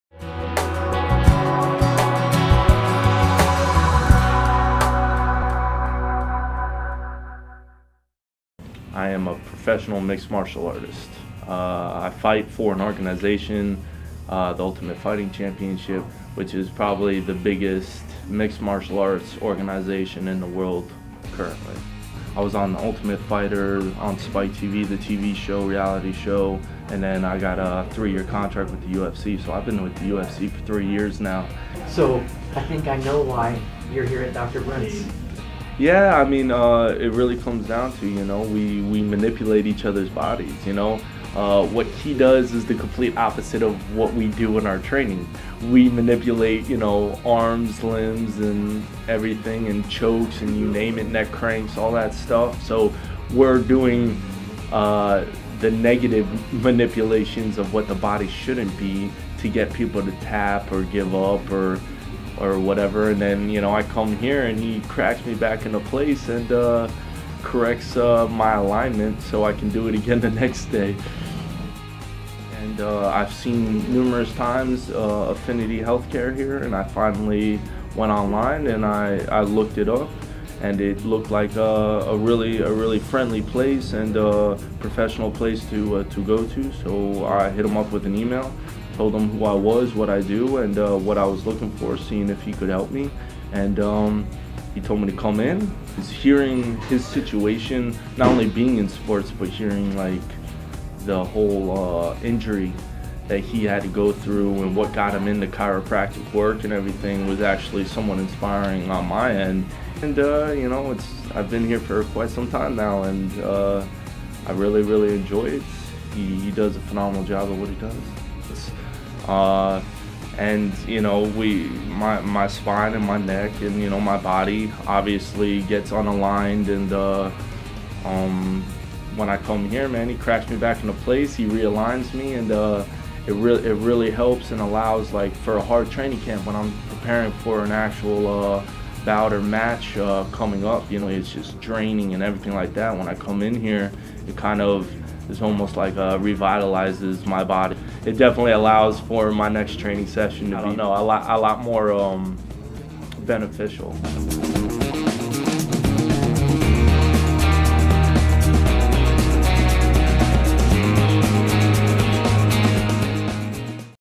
Professional MMA Fighter Ben Saunders Talks About How Chiropractic Helps
: Ben Saunders, a world famous, professional Mixed Martial Arts fighter, talks candidly, about the physical damage that MMA fighters do to their bodies and how important good Chiropractic care has been to his career.  In particular, he talks about how and why he chose Affinity Health Care and Dr. Brent Baldasare for his care.